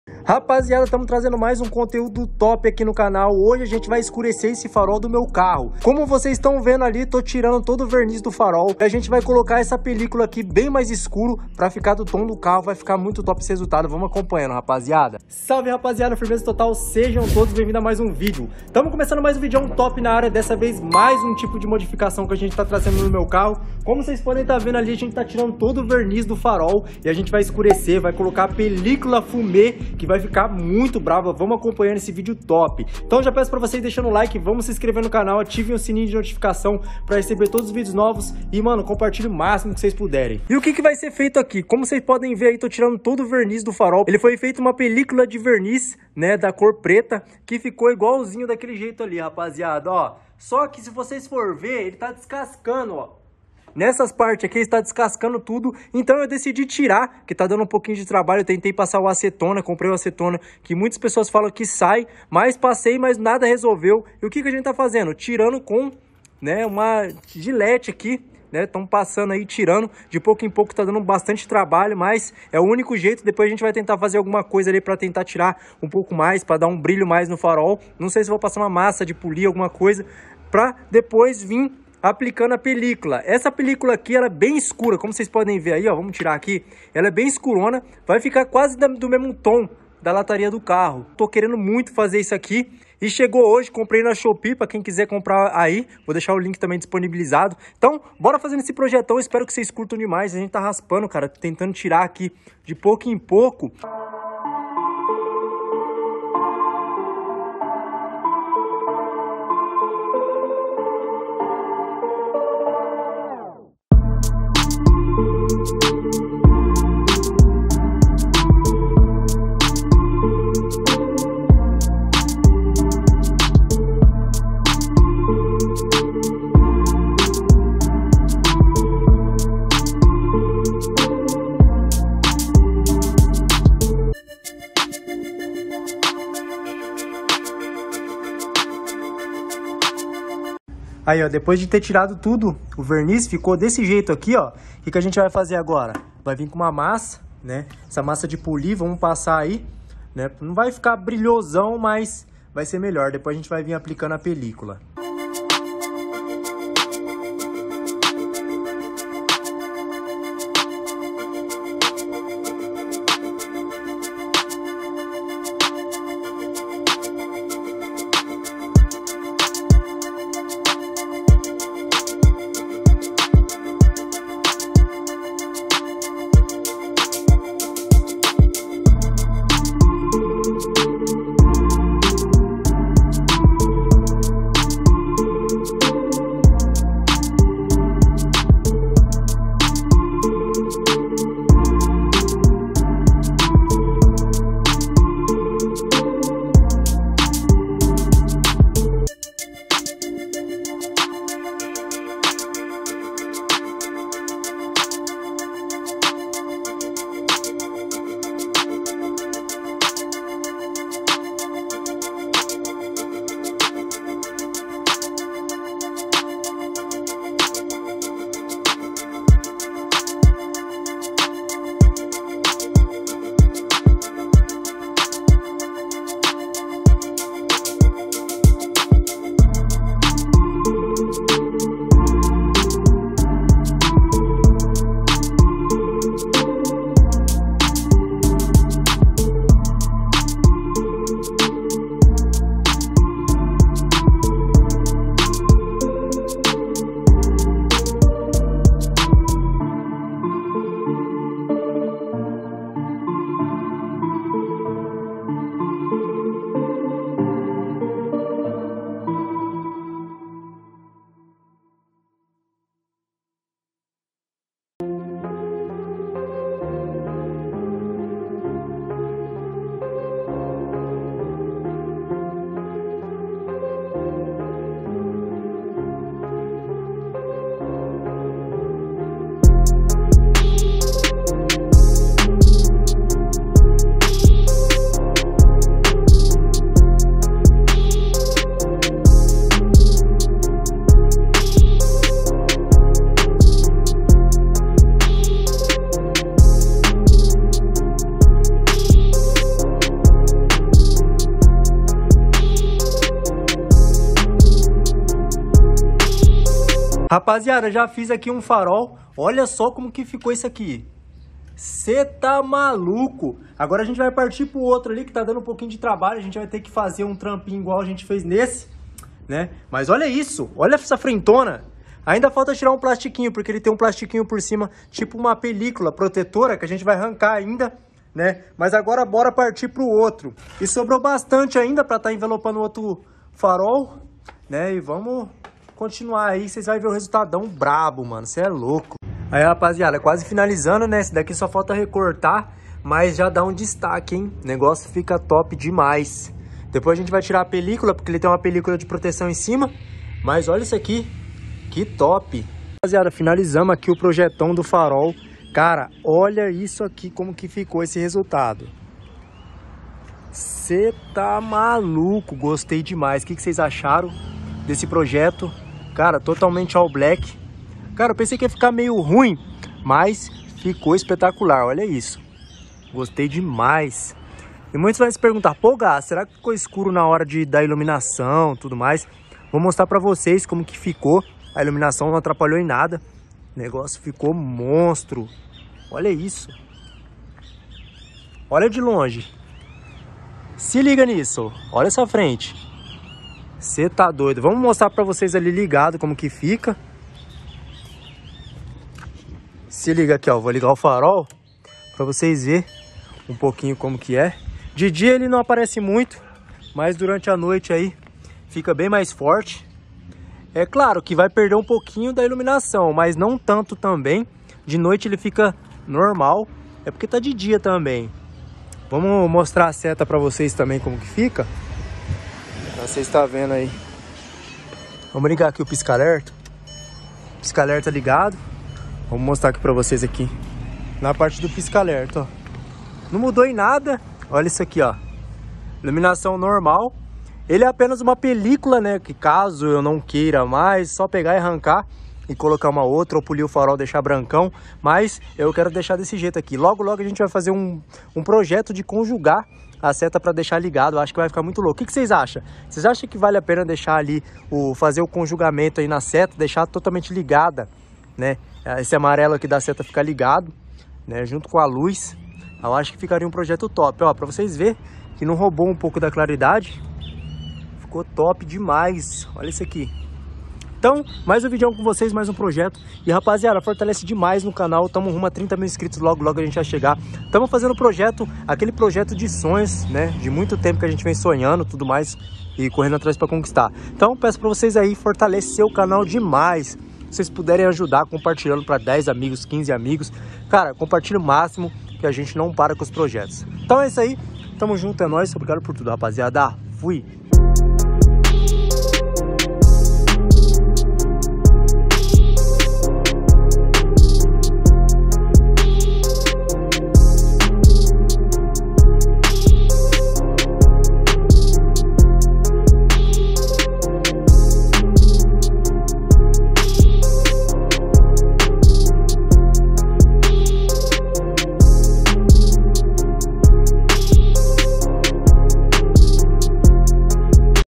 Fazendo Mascara Negra No Farol COMO FAZER
Bora fortalecer rapaziada , mais um projeto saindo do forno pra inspirar voces a montar os projetos do sonhos, então resolvi escurecer o farol do polo , oque acharam , achei que fico muito loko

► Carro : POLO SEDAN 2008
► Carro : VOYAGE QUADRADO ANO 85
► Carro : FIESTA SUPER CHARGE ANO 2003
► Carro : FORD KA 2009